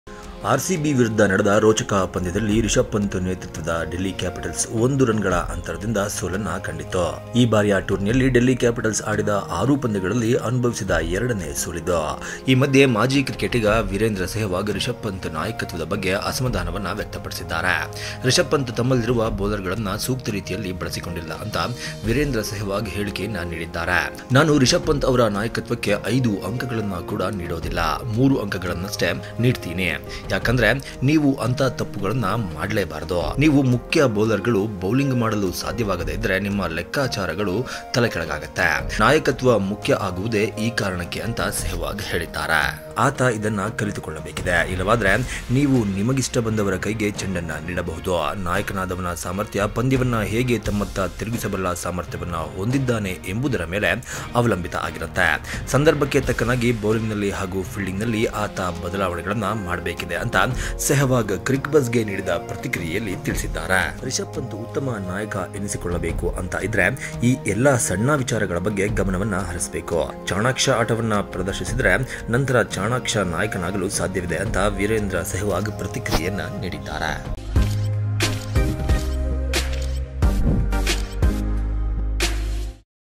Rishab Pant ನಾಯಕತ್ವದ ಬಗ್ಗೆ ಕಿಡಿಕಾರಿದ Virendra Sehwag | Oneindia Kannada
ಆರ್‌ಸಿಬಿ ವಿರುದ್ಧ ನಡೆದ ರೋಚಕ ಪಂದ್ಯದಲ್ಲಿ ರಿಷಭ್ ಪಂತ್ ನೇತೃತ್ವದ ಡೆಲ್ಲಿ ಕ್ಯಾಪಿಟಲ್ಸ್ ಕೇವಲ ಒಂದು ರನ್‌ಗಳ ಅಂತರದಿಂದ ಸೋಲು ಕಂಡಿದೆ. ಈ ಬಾರಿಯ ಟೂರ್ನಿಯಲ್ಲಿ ಡೆಲ್ಲಿ ಕ್ಯಾಪಿಟಲ್ಸ್ ಆಡಿದ ಆರು ಪಂದ್ಯಗಳಲ್ಲಿ ಅನುಭವಿಸಿದ ಎರಡನೇ ಸೋಲಿದು. ಈ ಮಧ್ಯೆ ಮಾಜಿ ಕ್ರಿಕೆಟಿಗ ವೀರೇಂದ್ರ ಸೆಹ್ವಾಗ್‌ಗೆ ರಿಷಭ್ ಪಂತ್ ನಾಯಕತ್ವಕ್ಕೆ ಅಸಮಧಾನವನ್ನು ವ್ಯಕ್ತಪಡಿಸಿದ್ದಾರೆ
Team India former cricketer Virendra Sehwag talk about Delhi capitals captain Rishabh pant captaincy against Royal Challengers Bengaluru
Oneindia kannada